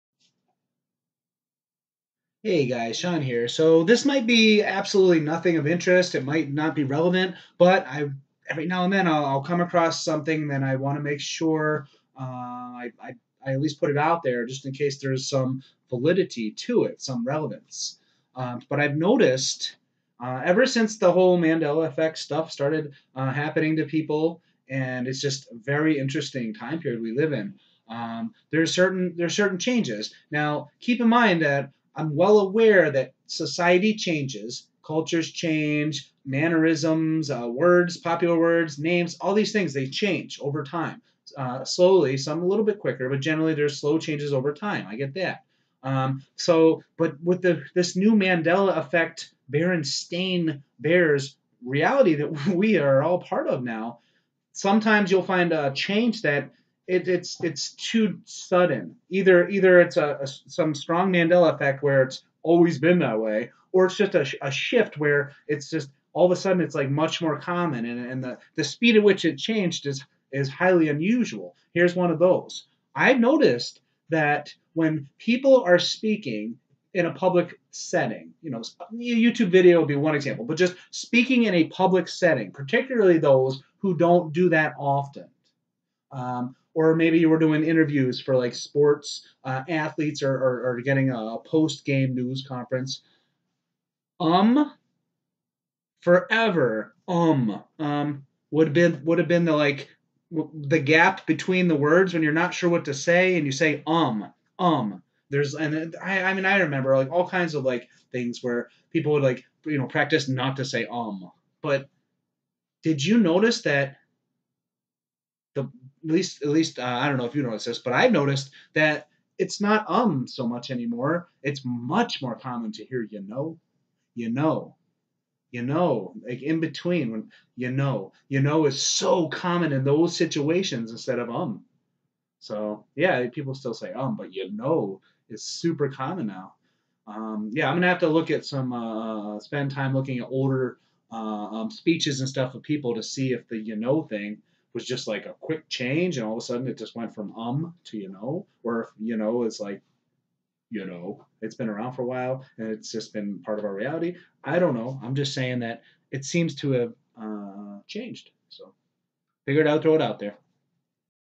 Have you Noticed this Subtle Mandela Effect | Berenstain Universe is Different
When someone would make a speech in public they would say "Umm" t times. Now, in that situation, it seems to almost always be "You know."